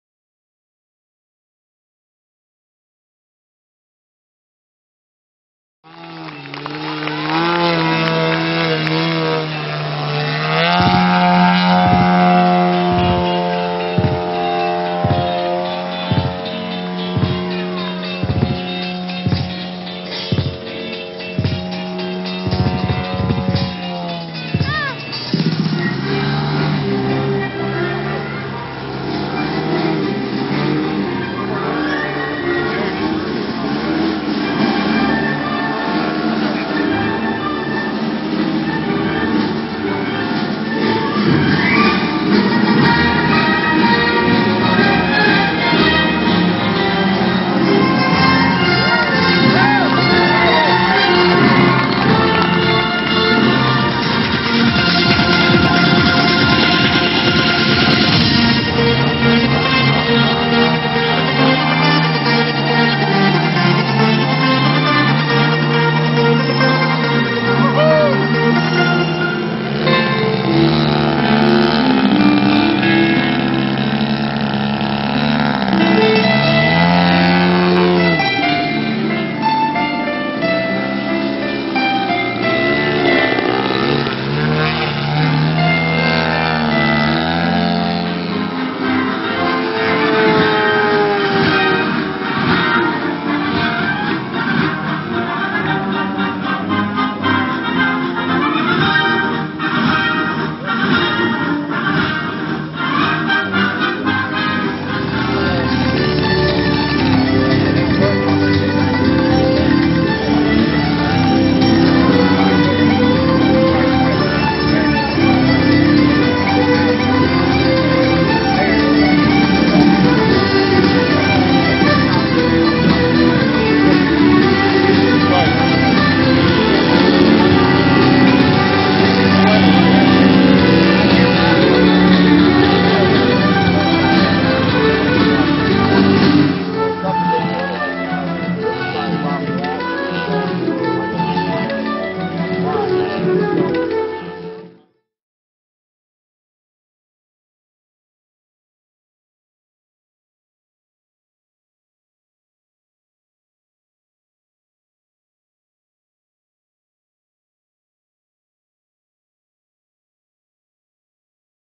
Highlights of the 2004 Don Lowe Masters
See Chip Hyde, Ivan Kristenson, Andrew Jesky, Jason Shulman, Ransom Fairchild and Peter Goldsmith pouring on the Smoke at the 2004 Don Lowe Masters to learn about the SmartSmoker Smoke system they were using.